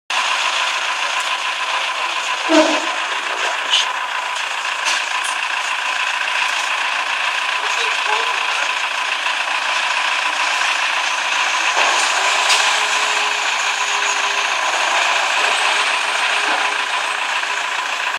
Sonderanfertigung Stapler von Ruf
Stapler Ruf Sonderanfertigung 8Tonnen Tragkraft bei 16Tonnen Eigengewicht